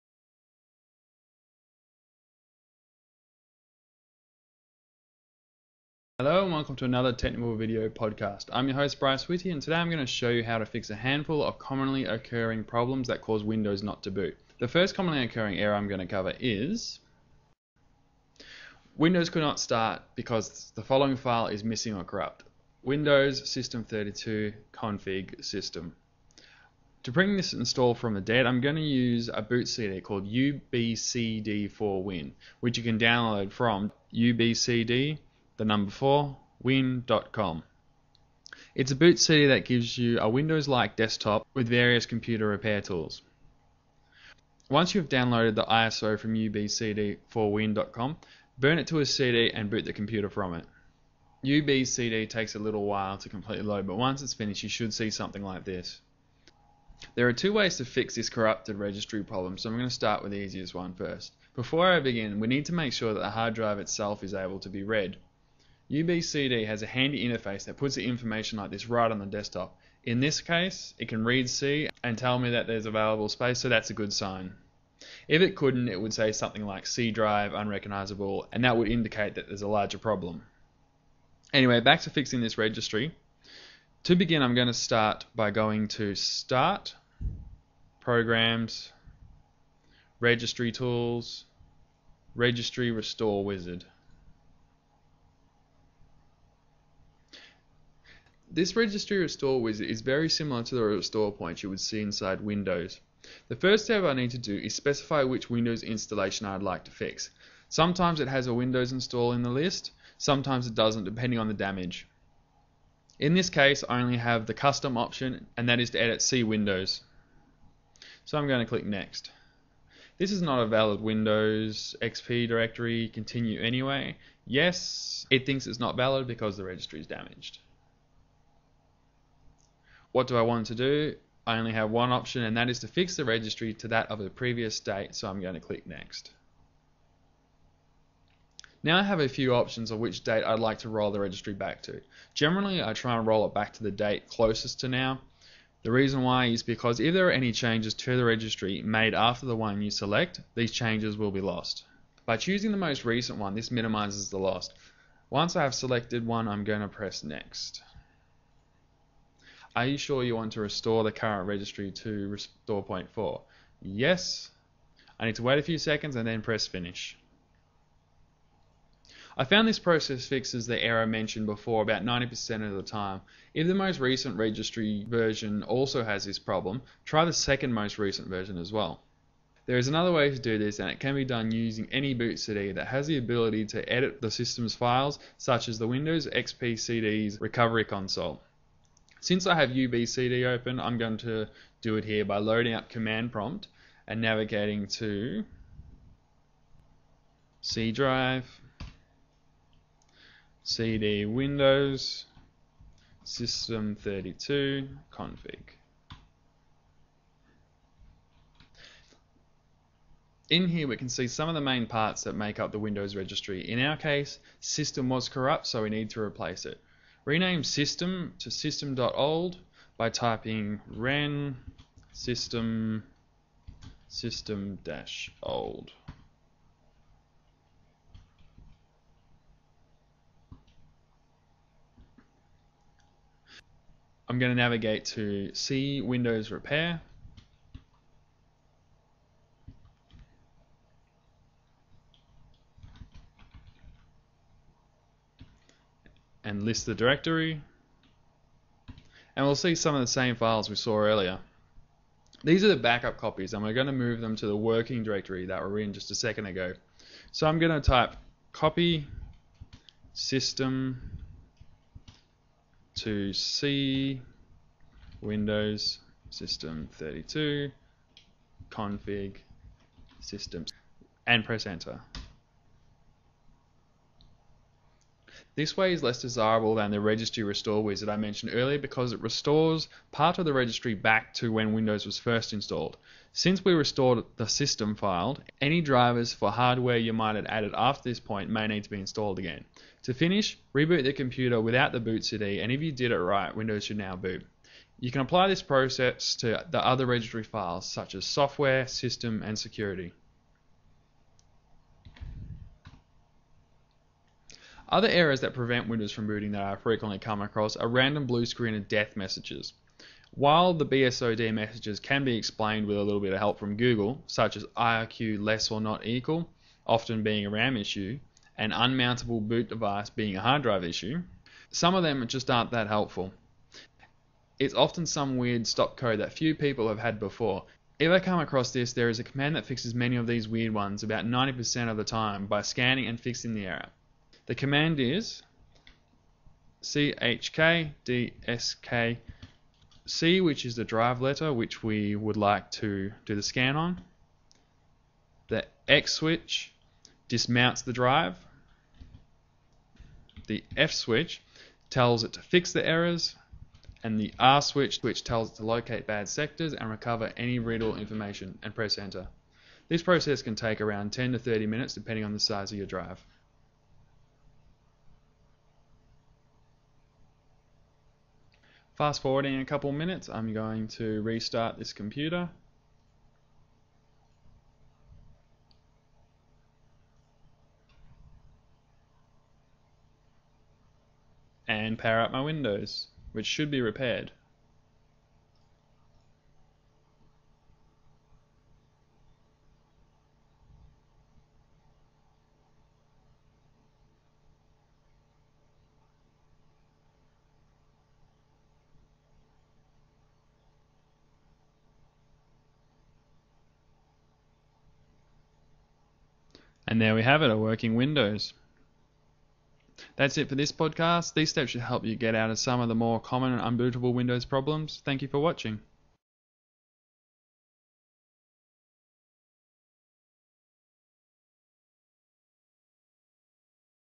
Recovering Windows from Common Errors that Prevent it From Booting - Technibble.com
If you are interested in starting a Computer Business or improving you existing one, check out
In this video podcast I will show you a few ways to repair some of the more common errors that cause Windows not to boot.